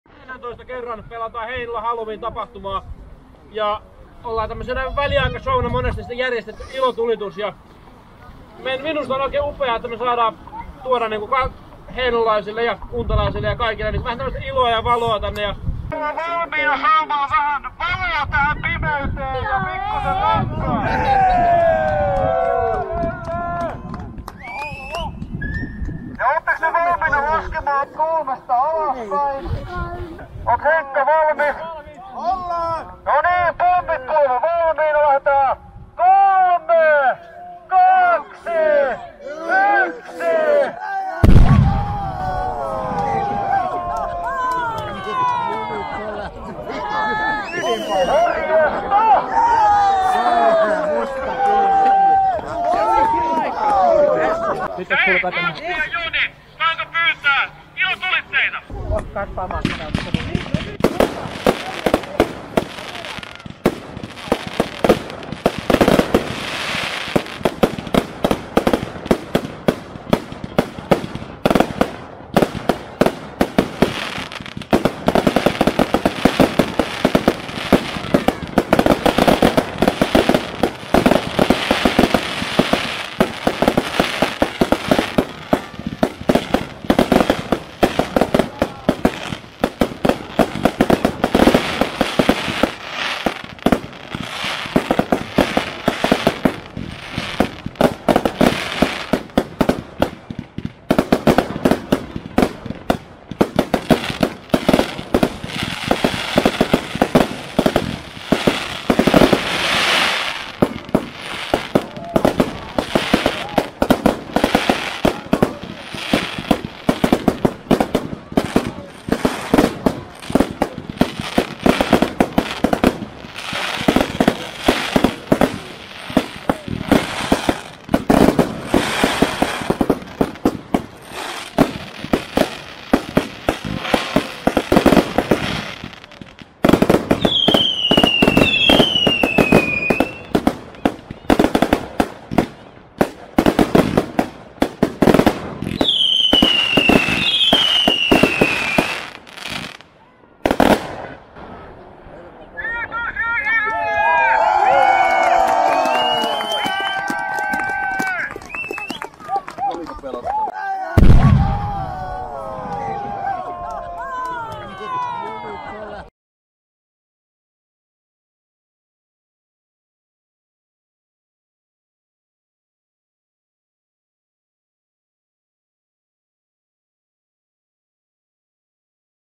Pommi-Henkka Heinola 2019
Heinolassa frisbeegolf tapahtuman yhteydessä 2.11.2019 oli lopussa myös ylöisölle erikoisviihdettä möljänpuistossa kun ns. pommi-Henkka, pommijätkät räjäyttivät näyttävän pommin. Noin 2sek ajan oli havaittavissa selvää lämpöä ja kirkasta valoa pimeydessä.